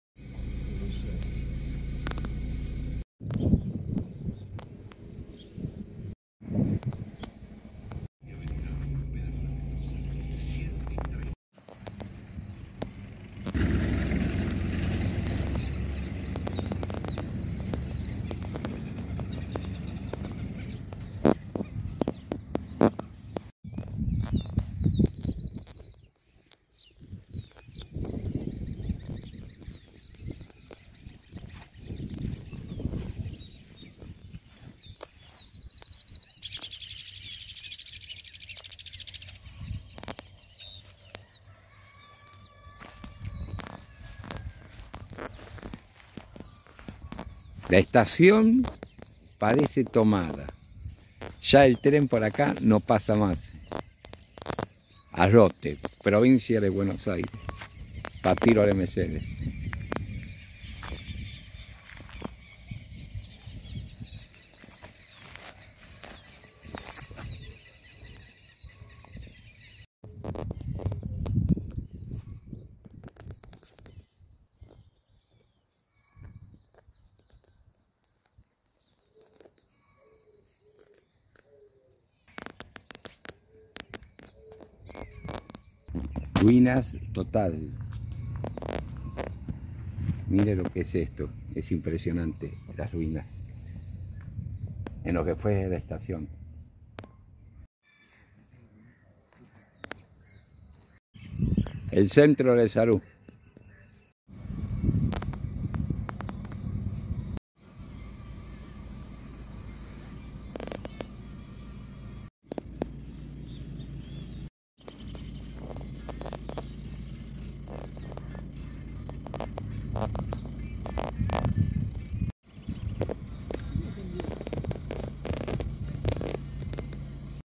AGOTE , PARTIDO DE MERCEDES PROVINCIA DE BUENOS AIRES....
Agote es una localidad de la Provincia de Buenos Aires, Argentina.
Ubicada en el partido de Mercedes, a 9 km de la ciudad cabecera del distrito y a menos de 2 km de Gowland.
Su población se incluye en los últimos dos censos, dentro de la localidad de Gowland. Cuenta con 1,738 habitantes (Indec, 2010), lo que representa un incremento del 35% frente a los 1,288 habitantes (Indec, 2001) del censo anterior.

INTENDENTE : JUAN VITARROZ
CODIGOS POSTAL : 6608
PREFIJO TELÉFONICO : 02324